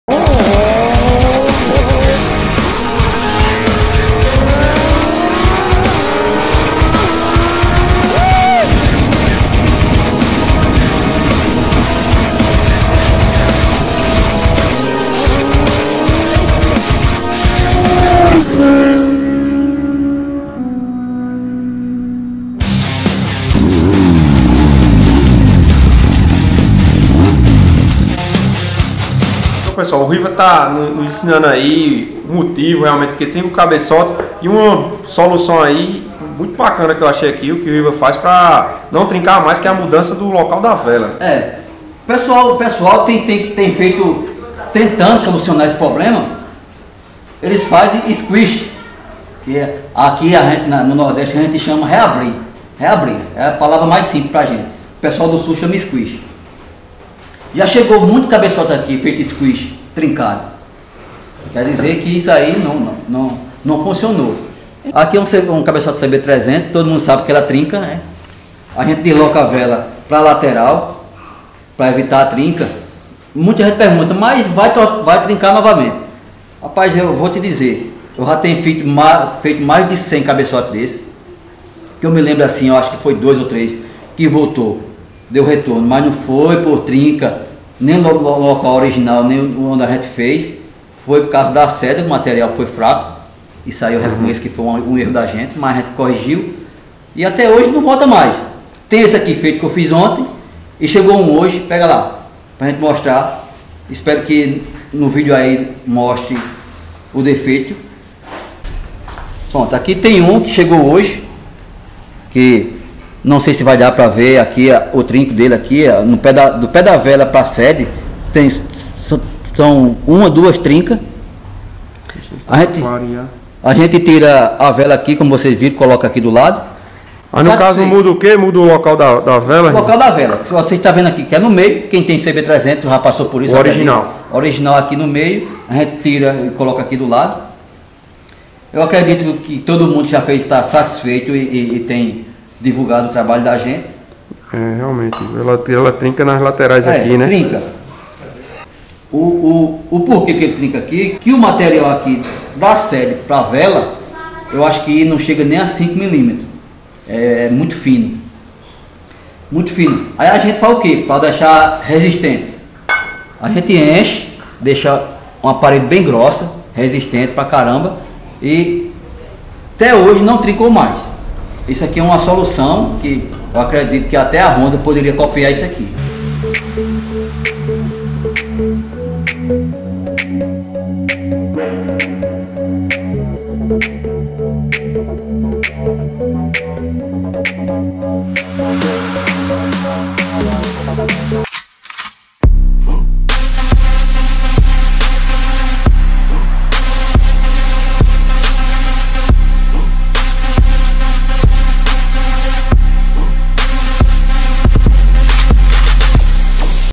Solução para o Cabeçote da CB300 não trincar mais...
Resolvendo de vez o problema do cabeçote da CB300